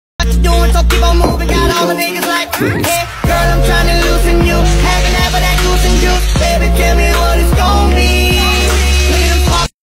Splash and Run!
The refillable water gun is the ultimate companion for outdoor water battles and summertime fun. Designed for hours of entertainment, this innovative water toy combines sleek design with practical functionality. Crafted from durable, lightweight materials, the refillable water gun ensures a comfortable grip for both children and adults.